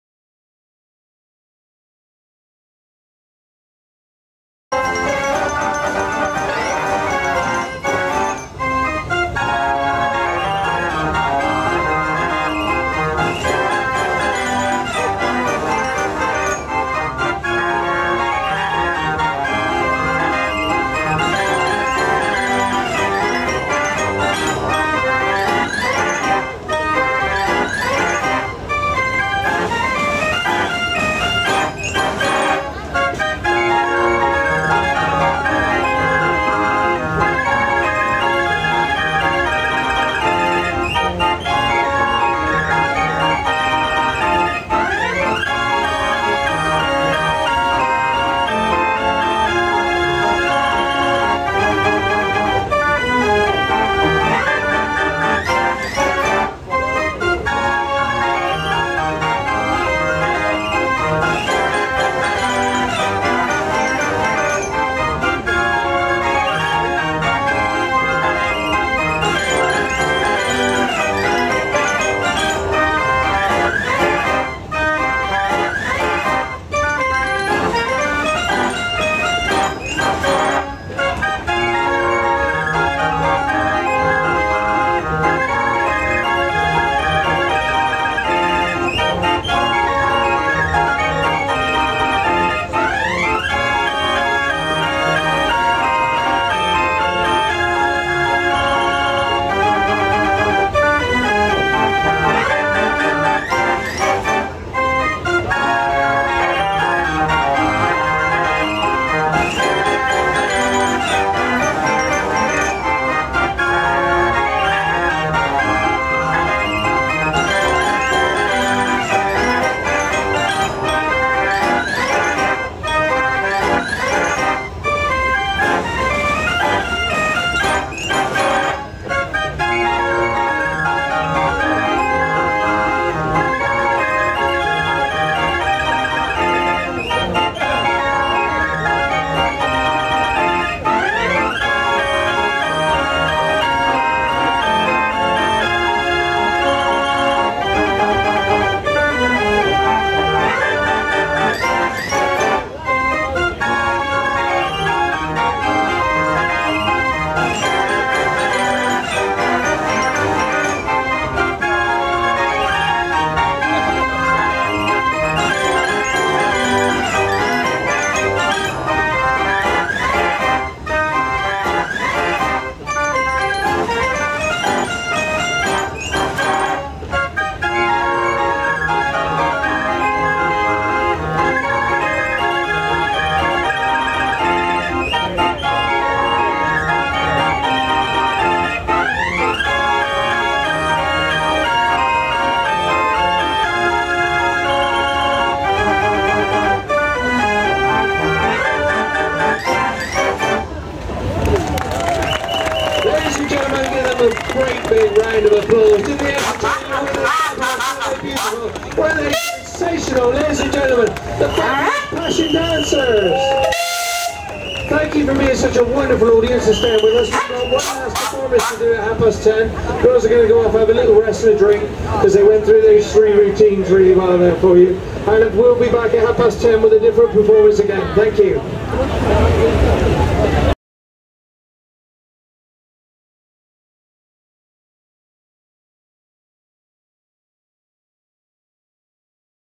The Passion Dancers - Great Dorset Steam Fair- England 2016
The Passion Dancers from Northampton on the stage in front of Richard Dean's family 90-keyless Dean Scenic Organ and bioscope show , at the 48th Great Dorset Steam Fair, at Tarrant Hinton ,England